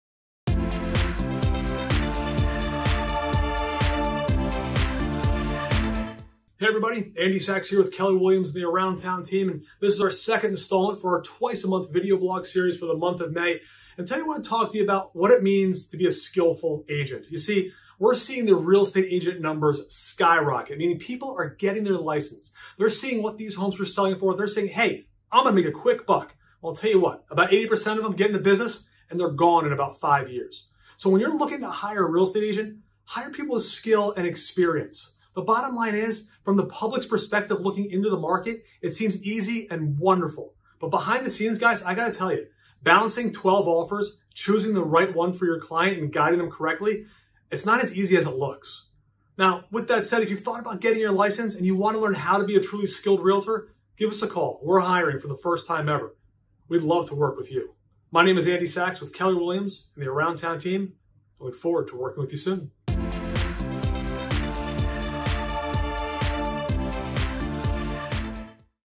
What It Means to Be a Skillful Agent
The number of real estate agents in the industry is skyrocketing. Many are getting their license, seeing what homes are selling for in today’s market, and think that it’s a way for them to make a quick buck.  In reality, around 80% of them enter and leave the business within about five years. If you’ve thought about getting your license, we have the resources to train you to become a truly skillful agent. To learn more, watch this short video.

Selling Your Connecticut Home?

ANDY SACHS   |   TEAM LEADER & BROKER
Top 1% Performing Agent & Team Nationwide
License # REB.0793345